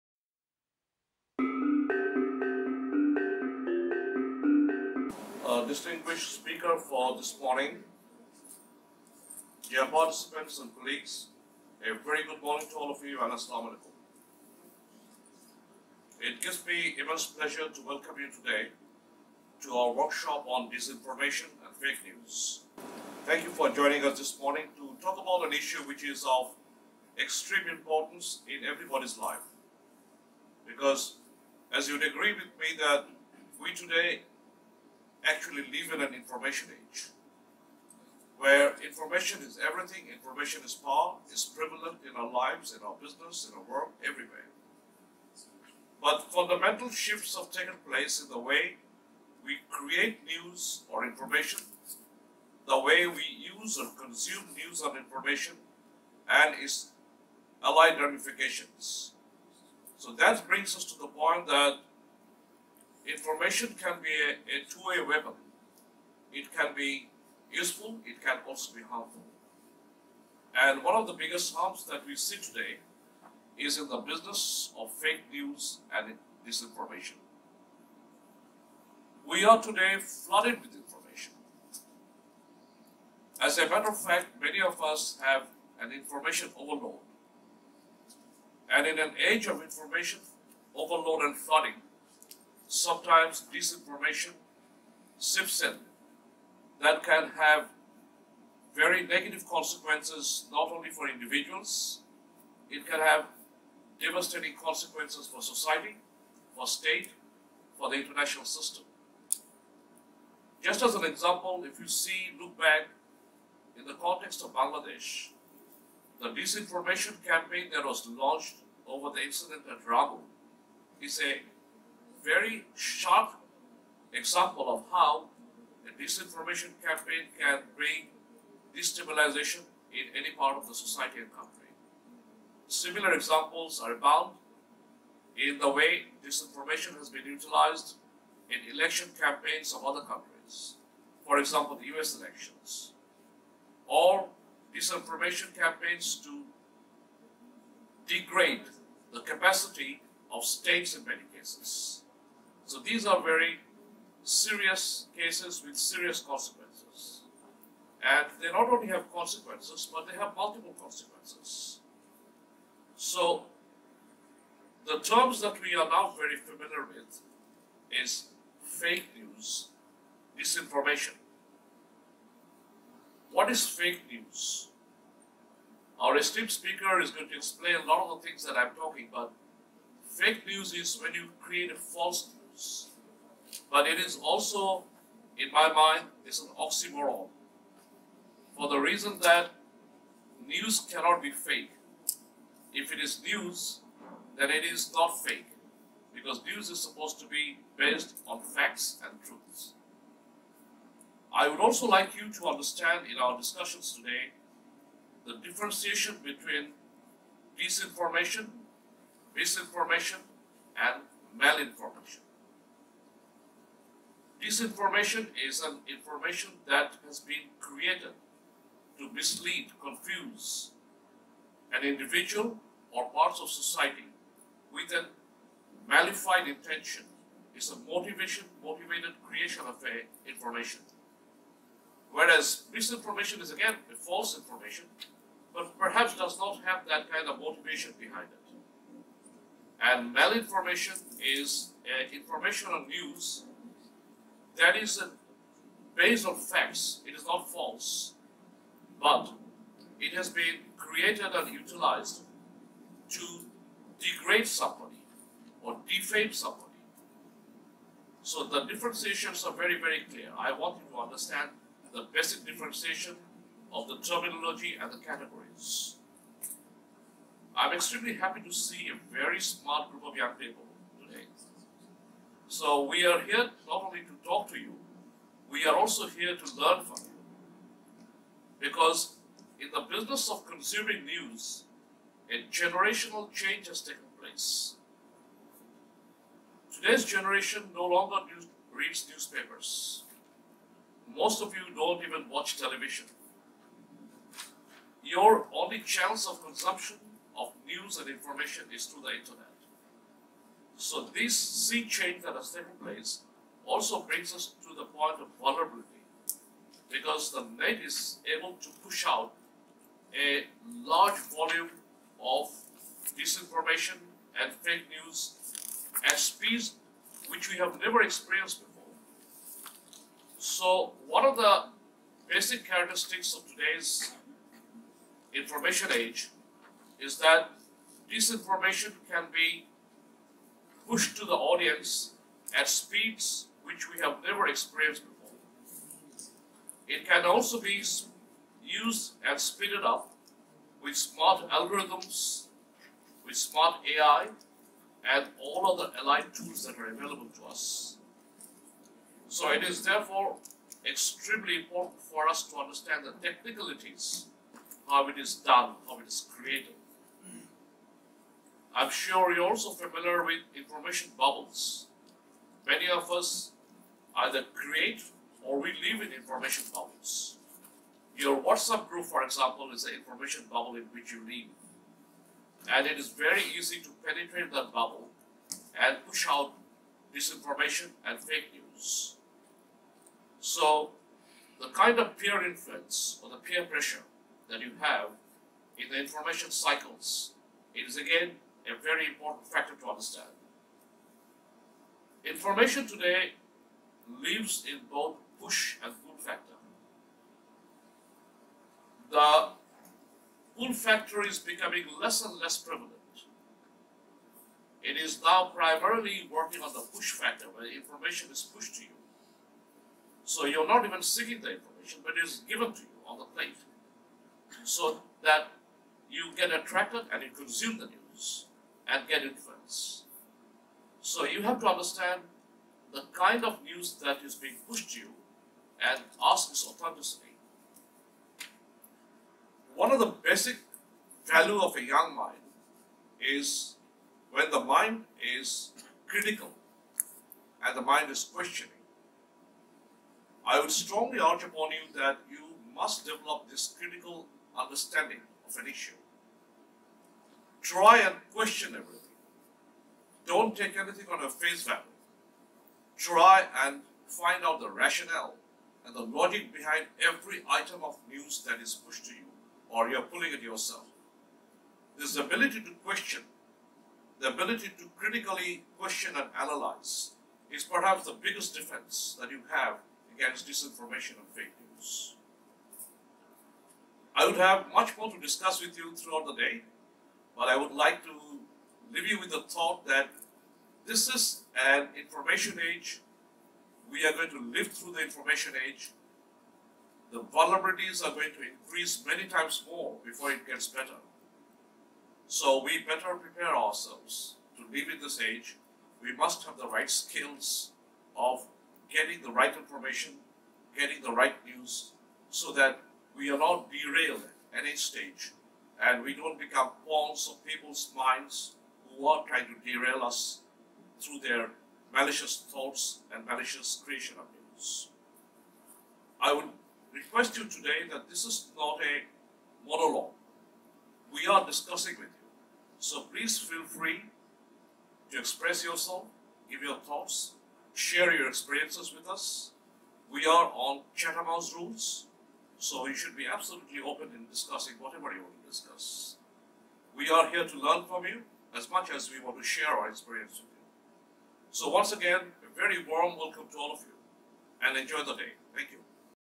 Opening Remarks of President, BIPSS at Disinformation & Fake News Workshop
Understanding the impact of Disinformation, Misinformation and Fake News is crucial in today's security landscape. As part of its endeavour to gain a deeper understanding into these issues, BIPSS recently organised workshops on Disinformation and Fake News targeted at youth participants drawn from various universities. The presentations were delivered by Ms. Ayesha Kabir, Consulting Editor of Prothom Alo English, Mr. Masud Chowdhury, Senior Lecturer, Department of Media and Communications at Independent University of Bangladesh.

The speakers highlighted the challenge emanating from disinformation and fake news in today's context. They also talked about the need to be discerning while using social media. The speakers also touched upon the growing threat of influence operations using social media and the need for policy formulation to tackle the challenge. BIPSS will continue to study this subject and organise more events in future.